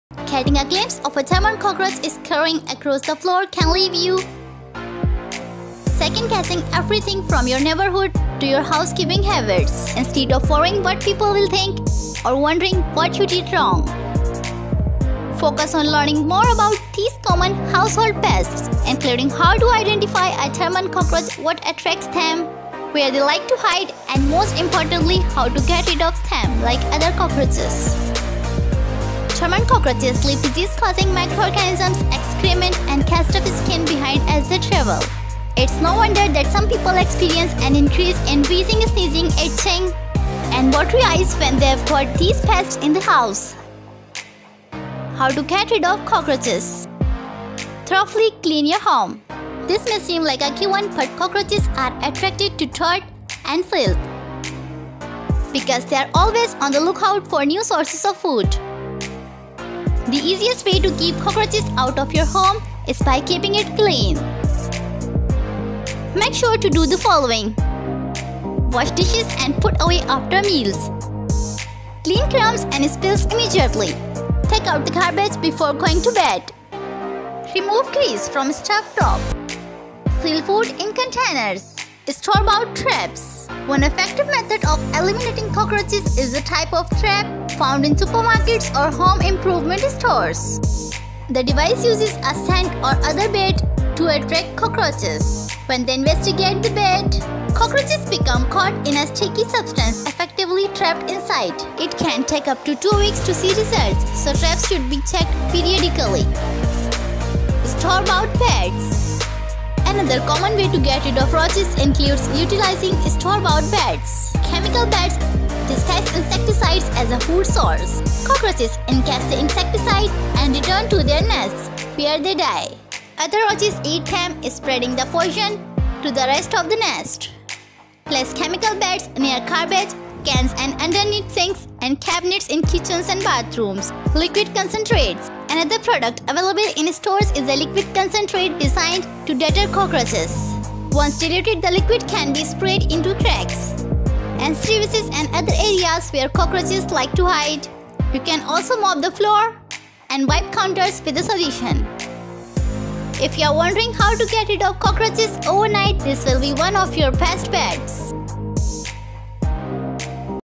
How to Get Rid of Cockroaches Permanently / Cockroaches in kitchen / Killer Spray
In this video i am share best tips for Get Rid of Cockroaches Permanently. Thank you so much,